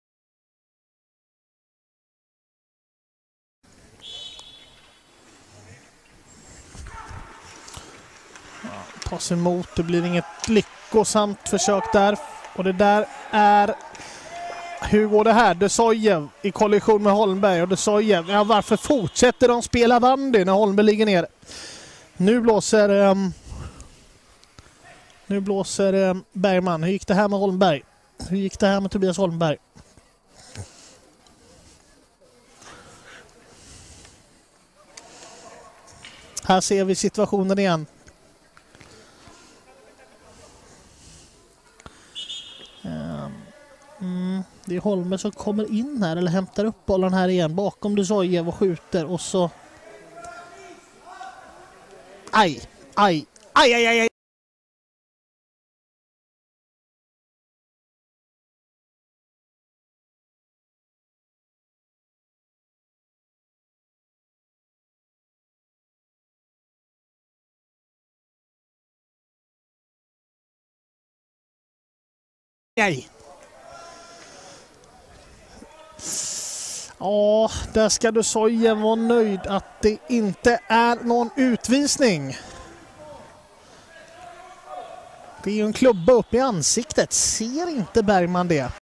Алан Джусоев«АИК»дисквалифицирован на 3 матча за удар игрока«Вестероса»1/4 финала/Элитсерия-Швеция/
Полузащитник Алан Джусоев«АИК»дисквалифицирован на 3 матча за удар игрока«Вестероса»Тобиаса Холмберга в 1/4 финала/Элитсерия-Швеция/